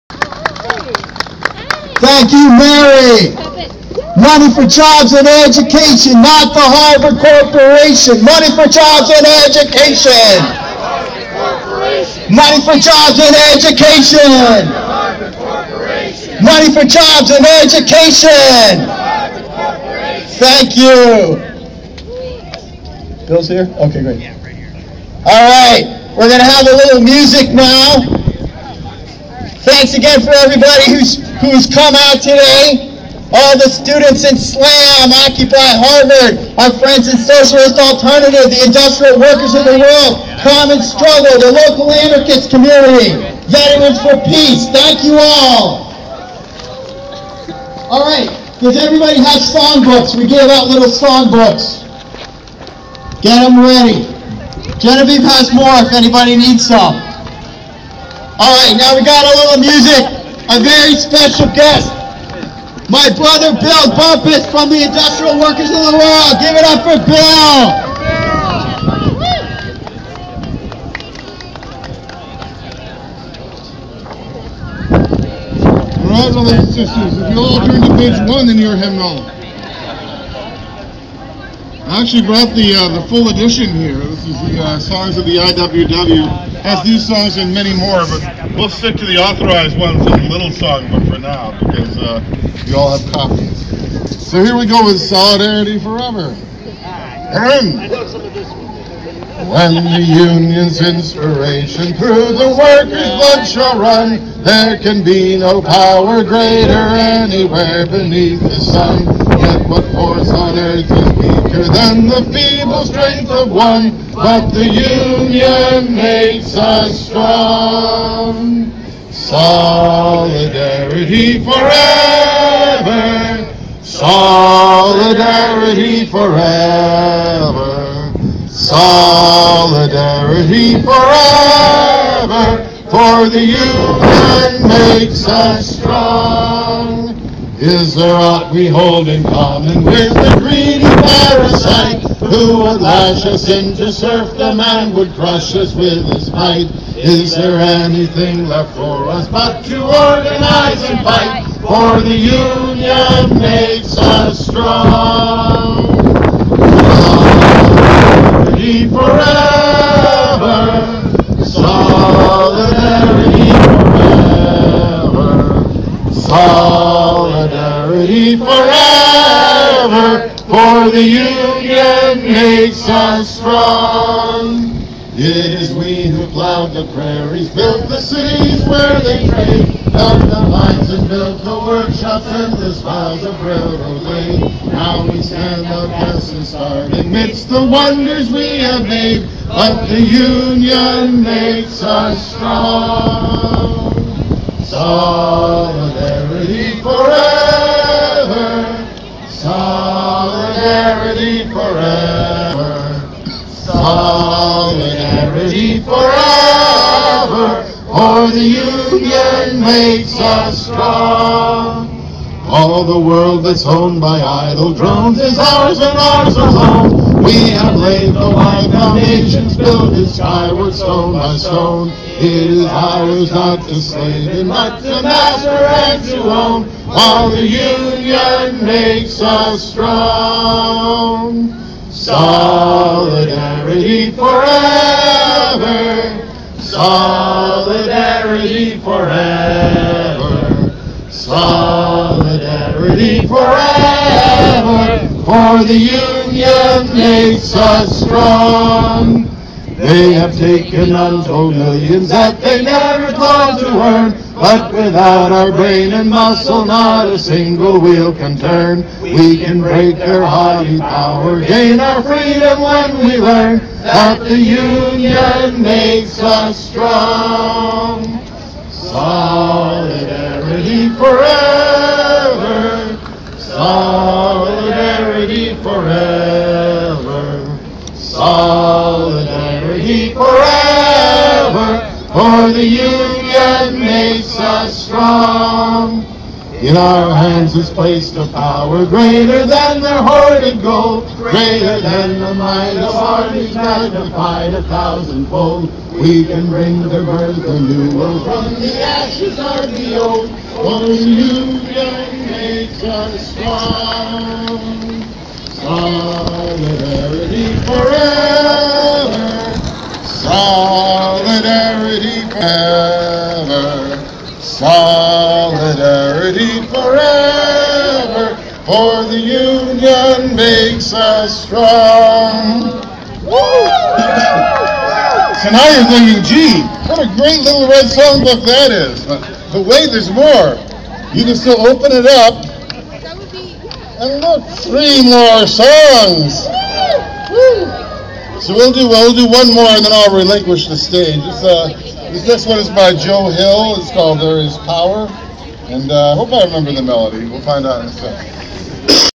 Speak Out Against Layoffs at Harvard Library (7 of 8)
"Speak Out Against Layoffs and Cuts in Library Services at Harvard," Science Center, noon, Mar. 27, 2012.